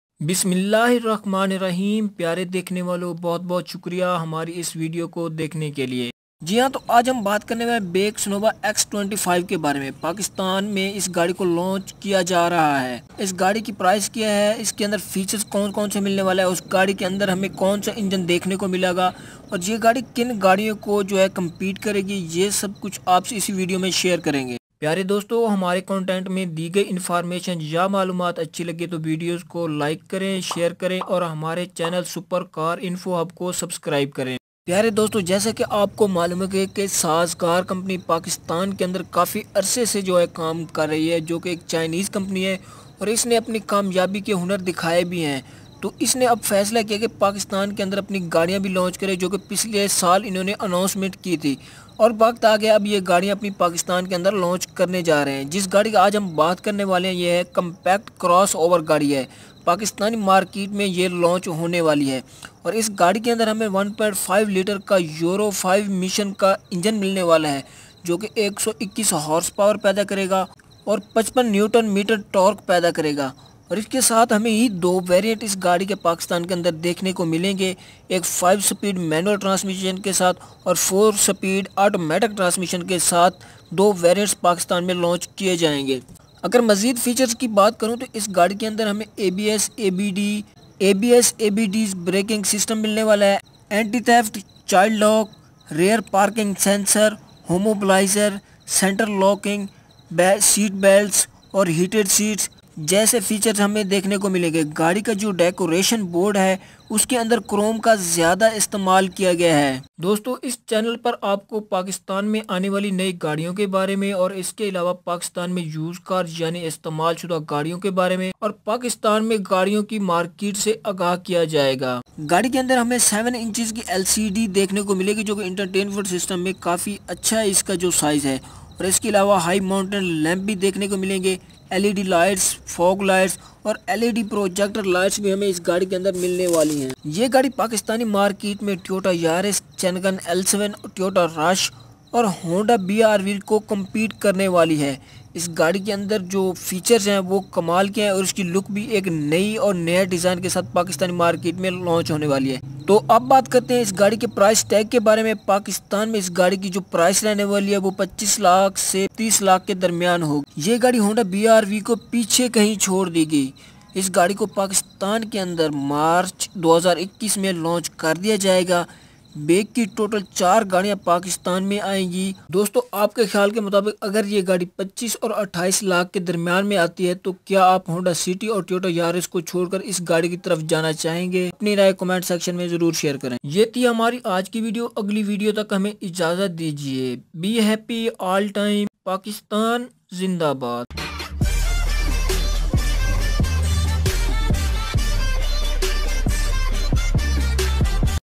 Baic Senova X25 Lounch To Pakistan | Price | Specs Features Details!
This is the new baick x25 car which lounching in pakistan soon.
this car in lounch in march month of 2021 in pakistan